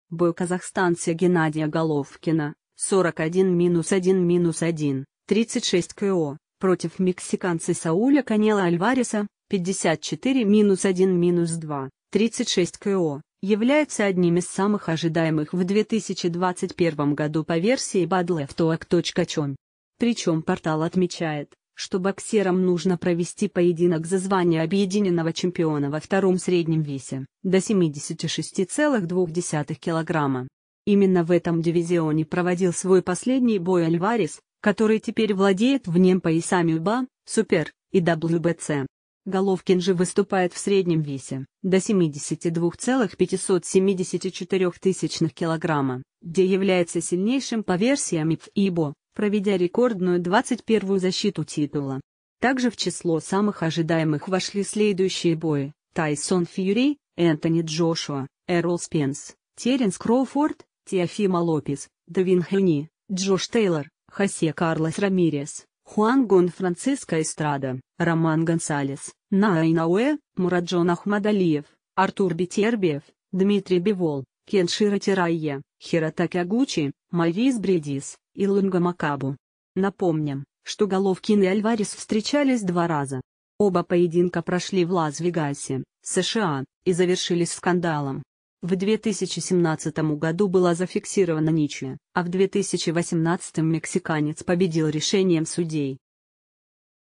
Третий бой Головкина с «Канело» вошел в список самых ожидаемых в 2021 году
Бой казахстанца Геннадия Головкина (41−1−1, 36 КО) против мексиканца Сауля «Канело» Альвареса (54−1−2, 36 КО) является одним из самых ожидаемых в 2021 году по версии Badlefthook.com.